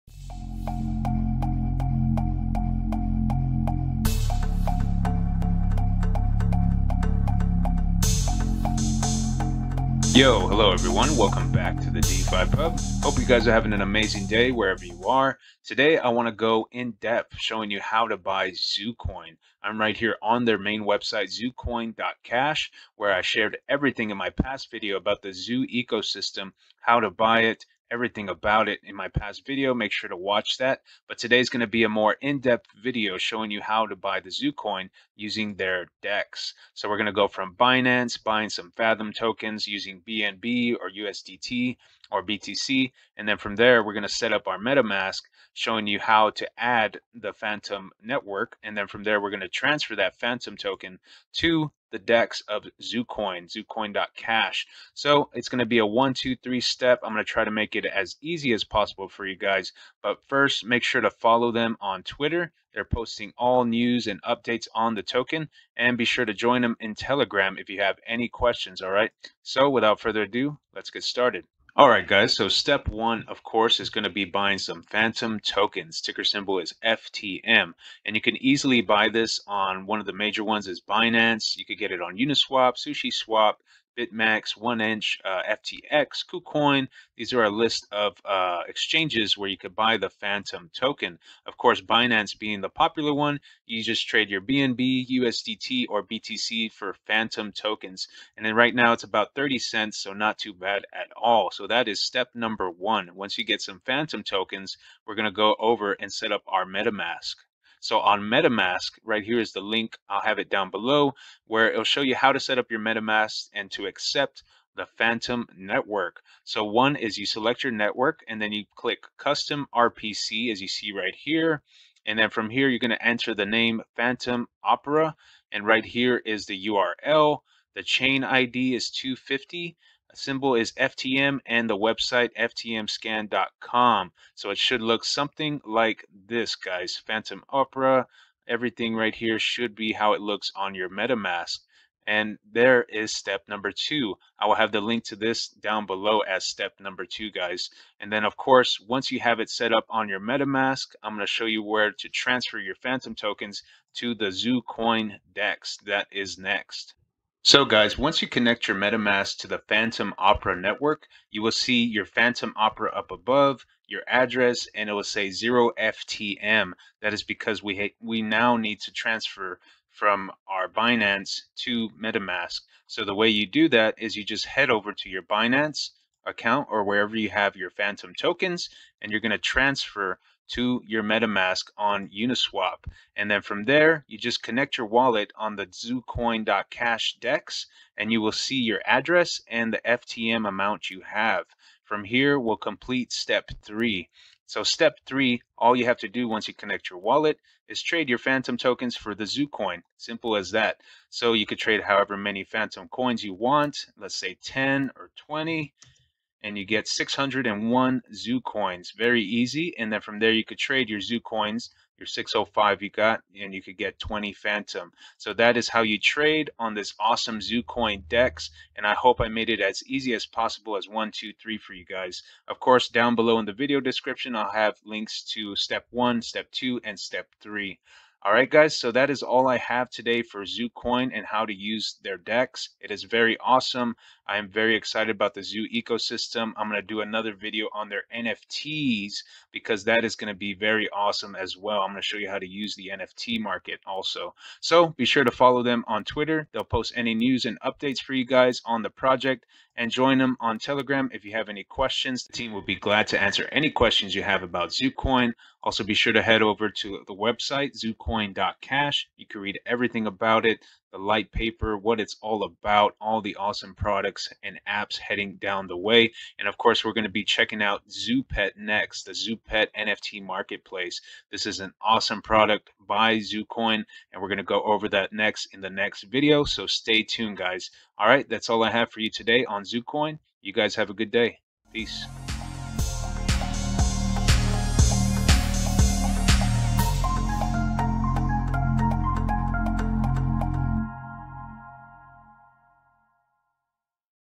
How to Buy ZooCoin using the ZooCoin Dex ($ZOO)
Today we'll be talking about ZooCoin - How to Buy ($ZOO) using our 3 step process. Links to all 3 steps will be down below with links on how to buy ZooCoin. Zoo is an ecosystem of decentralized applications powered by Fantom. Zoo has amazig features like its own DEX , Charts, NFT Marketplace, and Wallet.

Step #3 - Transfer FTM tokens to MetaMask and Connect Wallet to ZooCoin Dex

If you have any questions join on Telegram, the team would be glad to assist you.